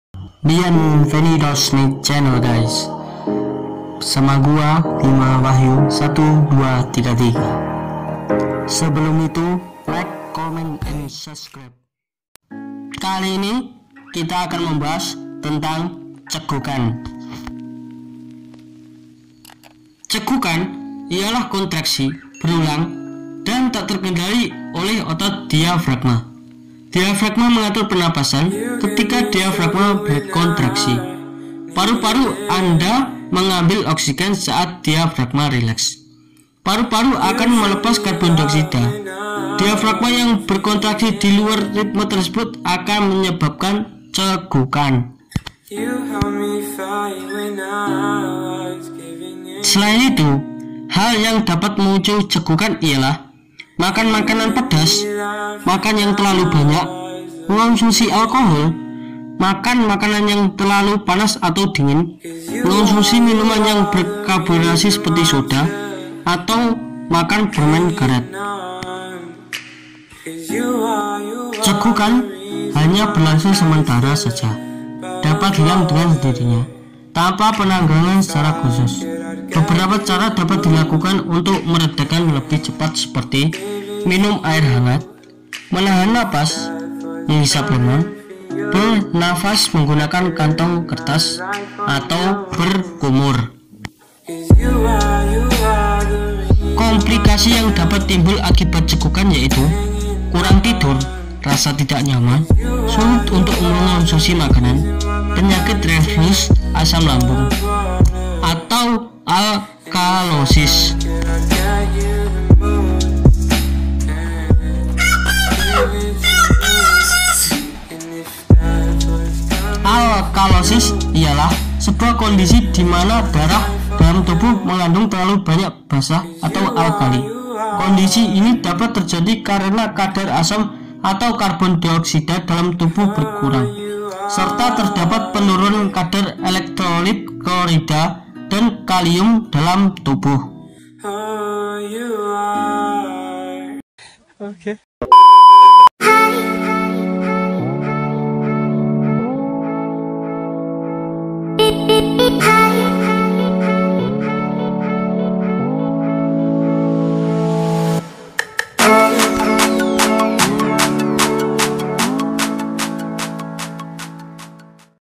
Pertanyaan masa kecil part 3 : kok bisa cegukan!!!!
Cegukan umum di seluruh Dunia,cegukan sejak lahir juga ada,tapi pernah ngak kamu mikir pas kecil kok bisa ya cegukan kayak gimana proses nya hahahaha Iya kan! Terus Dari besar juga Kamu belum paham apa itu cegukan gimana prosesnya ??? Yuk simak penjelasan berikut ini 🍃🍃🍃🍃🍃🍃🍃🍃🍃🍃